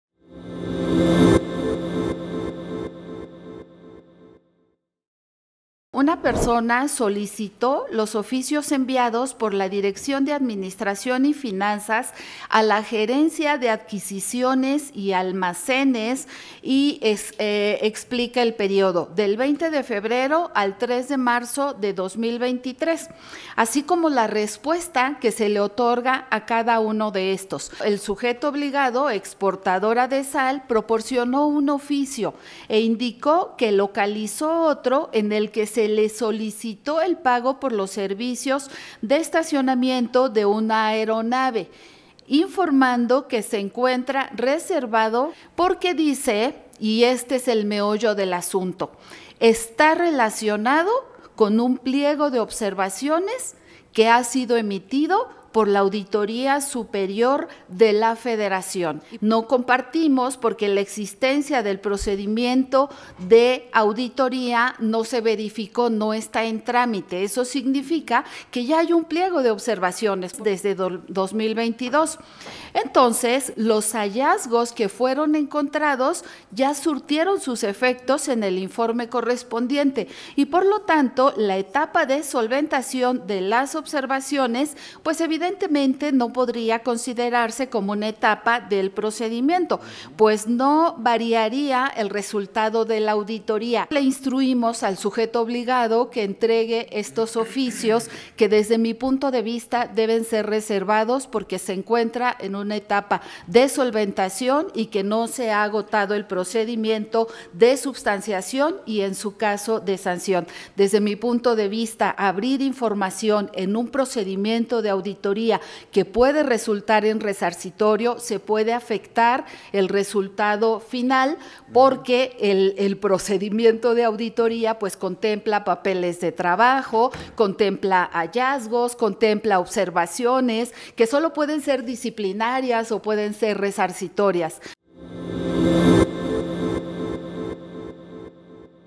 ElINAIteInforma JRV Exportadora de Sal debe proporcionar a una persona un oficio que fue...
Exportadora de Sal debe proporcionar a una persona un oficio que fue clasificado como reservado